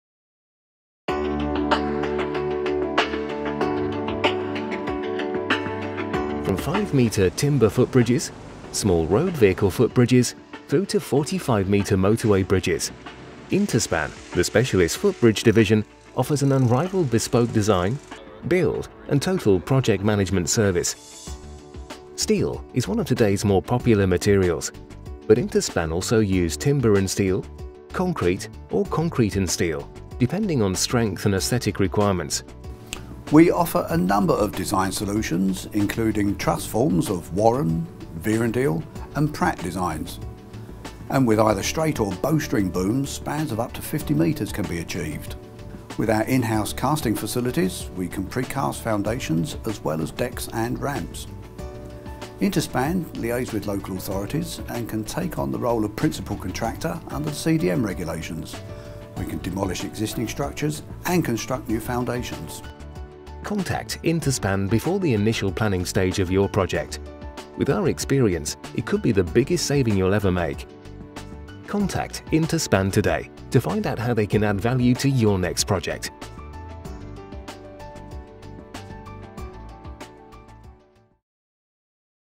Topbond PLC - Intaspan Footbridge Design and Build
Intaspan, the specialist footbridge division of Topbond plc, offers a complete footbridge design and build service.

Footbridges can be constructed in a range of materials, depending on client requirements, the location and function of the footbridge and, of course, the available budget.Steel is a popular option and we can offer a number of design options – truss formation of Warren, modified Warren and Vierendeel designs with either straight or bow string top booms – spans of up to 40m can be achieved. Steel beam bridges and plate girders can be designed and constructed, usually on a bespoke basis.

Other options include timber and concrete, or a combination of materials – for example steel and timber are frequently used to combine the strength and durability of steel with the aesthetic beauty of wood

Intaspan specialise in a total project management service, including design independent checking, AIP forms, general arrangement drawings, foundation construction, demolition of existing structures, installation and associated works.  We can assist with planning and  undertake necessary liaison with statutory undertakers, local authorities and other bodies and can undertake the role of Principal Contractor under CDM Regulations.